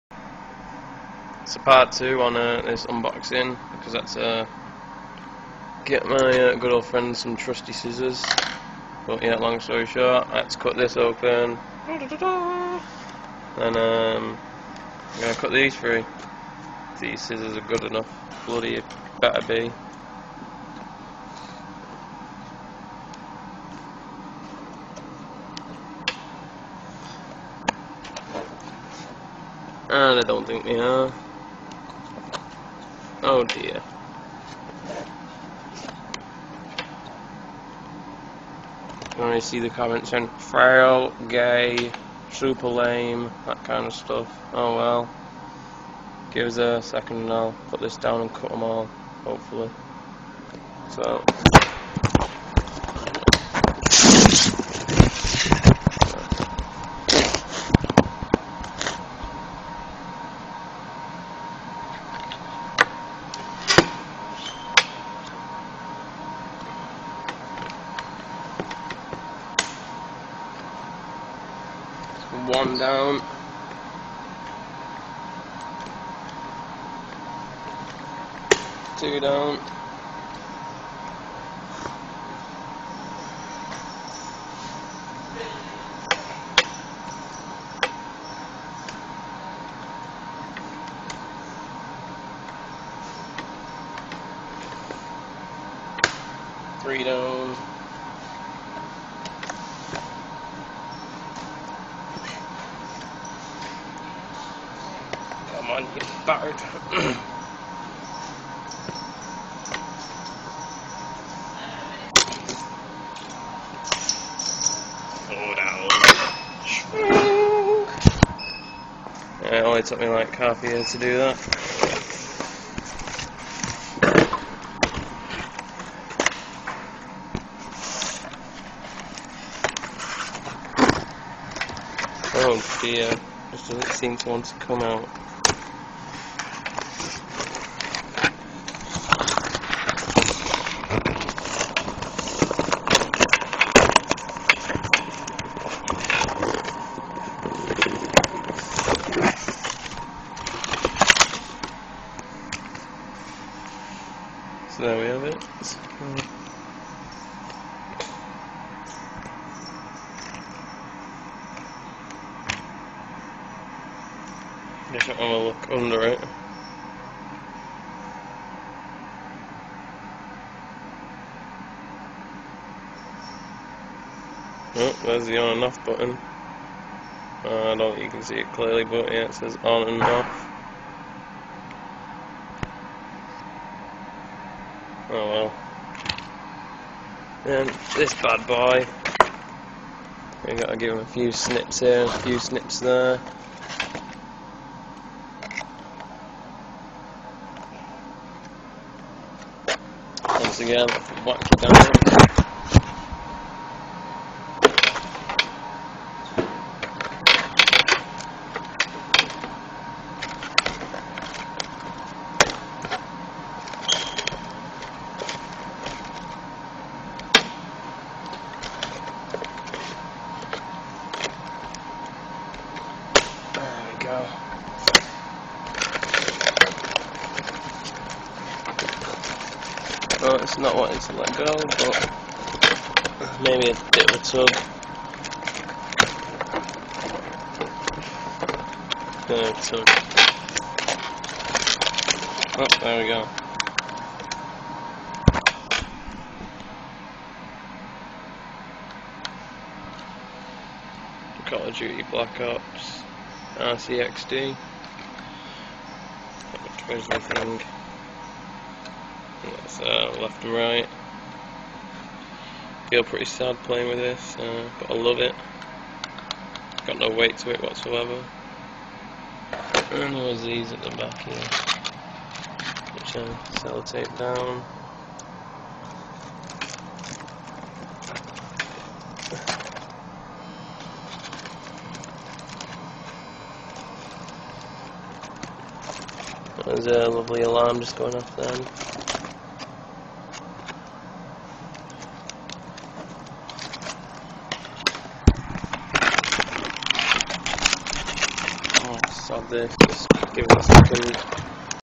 YouTube - Call Of Duty Black Ops Prestige Edition Unboxing! (Part 2)
Here's part 2 of the unboxing of prestige edition Call of Duty Black Ops.